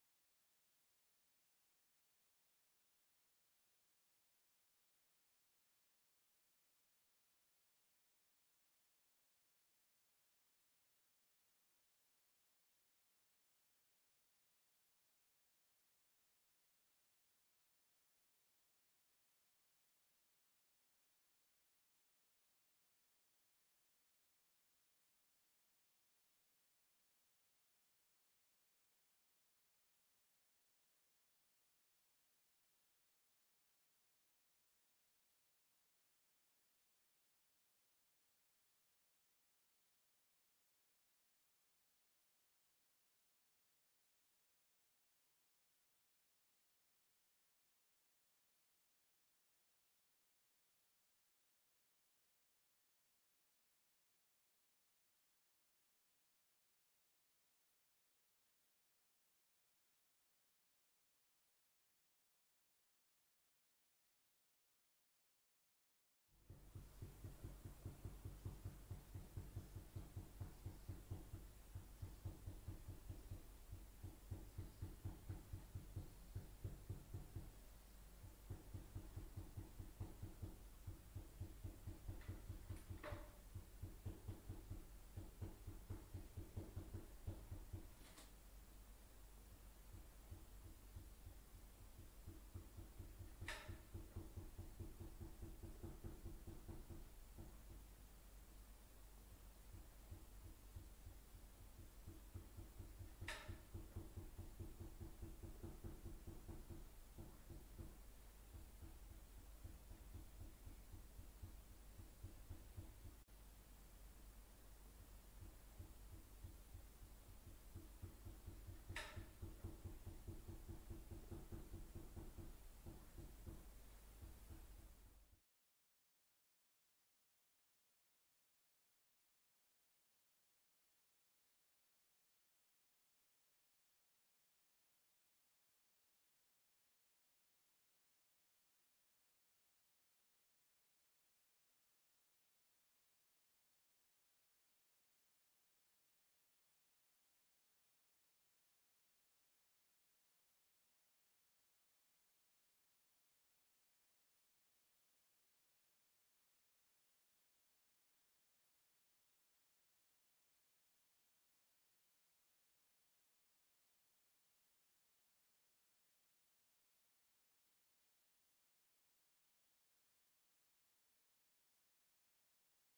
Making an Ink Rubbing | Explore with Us | Kelvingrove | Burrell Collection
Abstract art impressions inspired by The Burrell at Kelvingrove: Collecting Chinese Treasure Exhibition.

This contemplative film demonstrates the ancient art of making an ink rubbing from a textured surface.

The idea was inspired by the lustre of beautiful objects in this wonderful exhibition. Taking direct impressions from priceless artefacts or our own personal treasures obviously isn't acceptable. Here we see how everyday mundane objects with texture, like chopping boards offer scope to create beautiful abstract art pieces that are also inspirational.